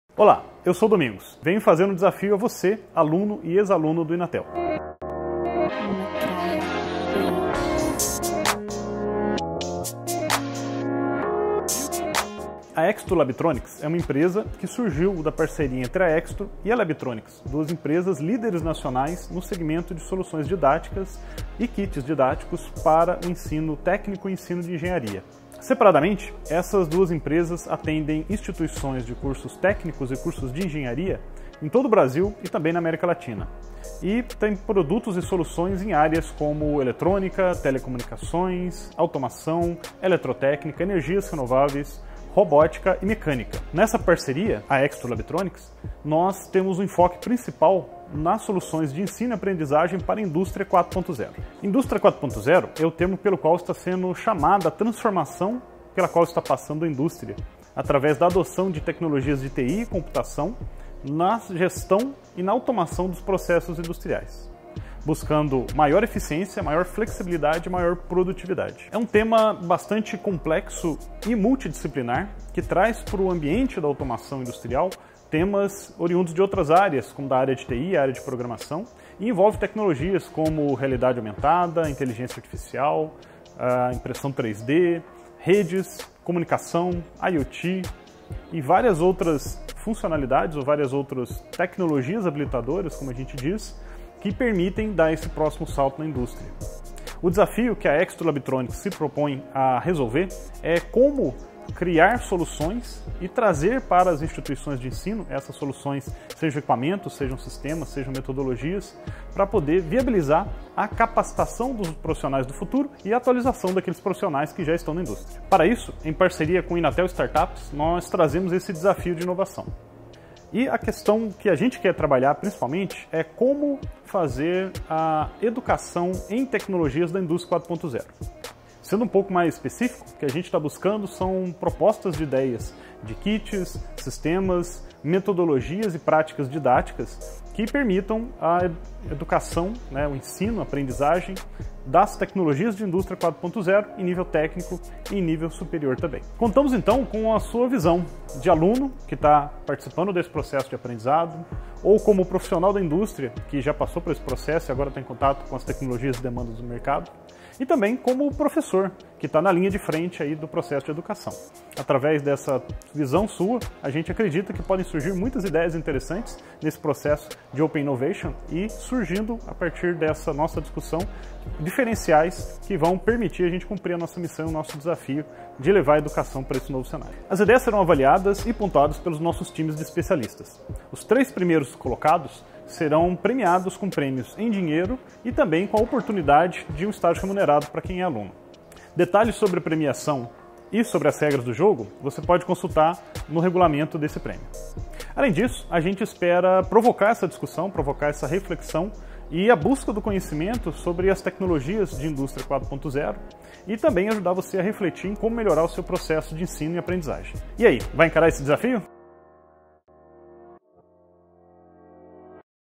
Inatel | Desafio de Inovação 2021: Exsto Labtronix
Saiba o que é o Desafio da Inovação 2021: Exsto Labtronix!
Como fazer a Educação de Tecnologias para a Indústria 4.0?

:: Inscrições 18/novembro a 21/fevereiro.